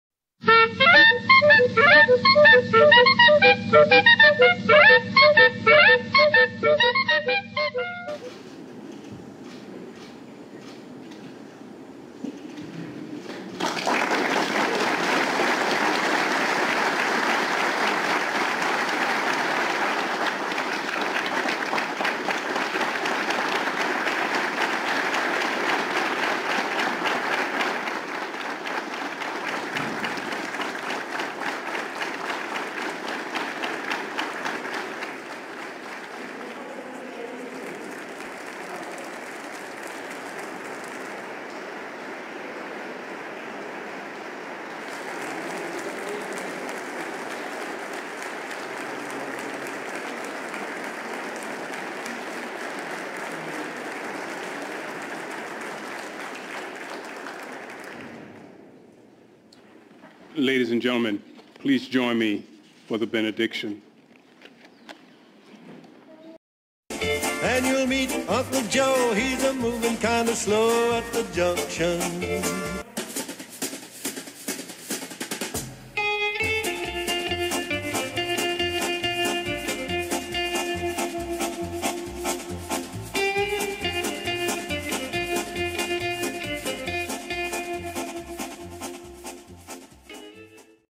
President Biden Walks Off Stage During Medal of Honor Ceremony
President Joe Biden appeared lost again as he abruptly wandered off stage during a Medal of Honor ceremony at the White House, leaving Captain Larry Taylor alone on stage to listen to the closing benediction of the ceremony.
Let's Go Brandon!